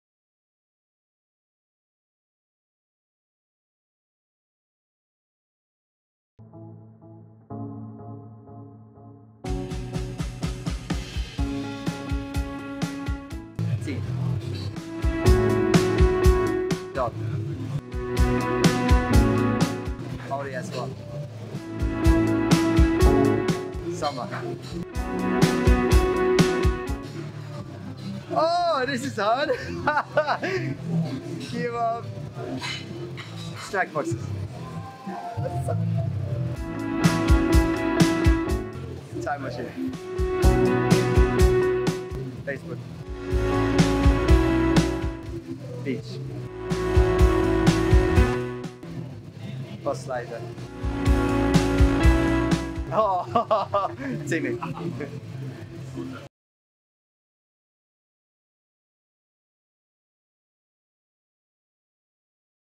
Quick fire questions with Andreas Bakkerud
We sat down with Monster Energy RX Cartel's Andreas Bakkerud
 to find out the answers to some not-so-serious quick fire questions...